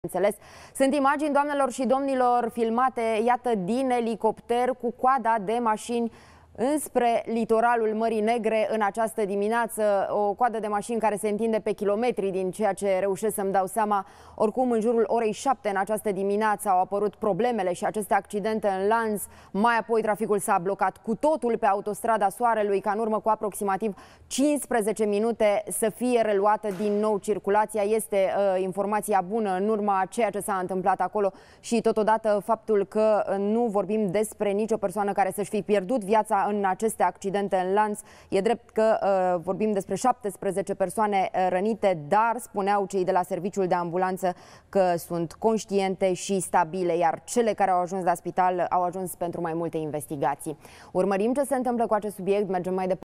Imagini din elicopter cu carambolul de pe Autostrada Soarelui. Peste 50 de mașini și 146 de persoane
Imagini filmate din elicopter arată coadă de mașini întinsă pe kilometri întregi, în urma carambolului cauzat de accidente în lanț, pe drumul către litoral, mai exact pe Autostrada A2, între km 172 și 183.